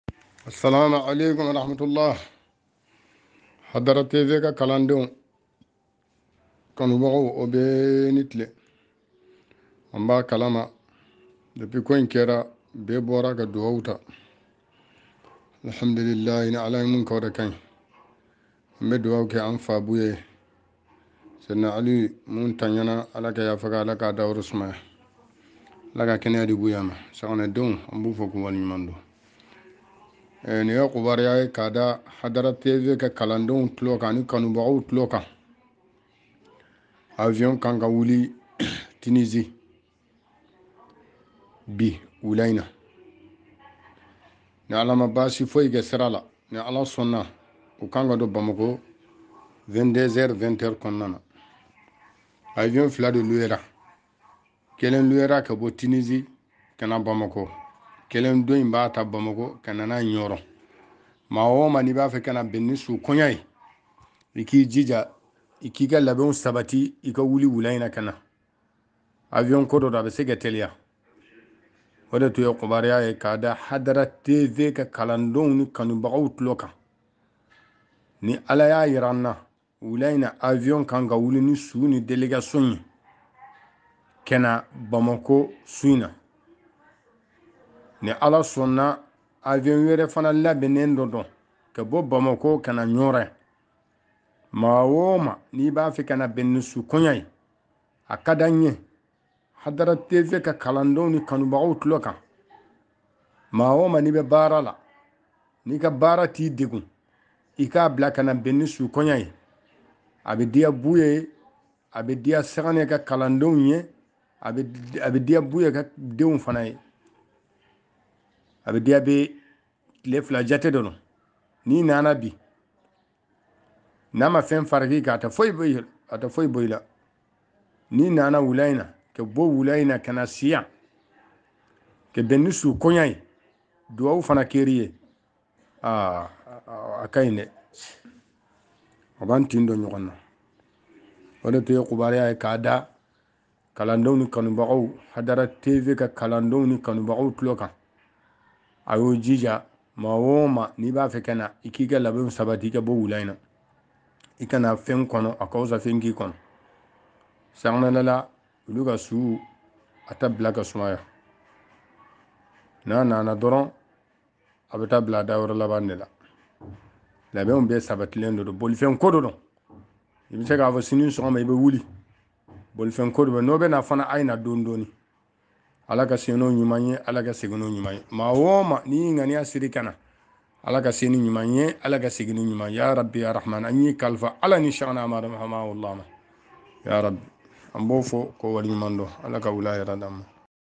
Le décollage de l'avion pour l'acheminement du dépouille de Bouye Seydina Aly en cours
hadra_actu_décès : Le décollage de l'avion pour l'acheminement du dépouille de Bouye Seydina Aly de Tunisie vers le Mali est en cours et l'arrivée est prévue vers 21h a 22h inchallah. Les funérailles auront lieu à Nioro du Sahel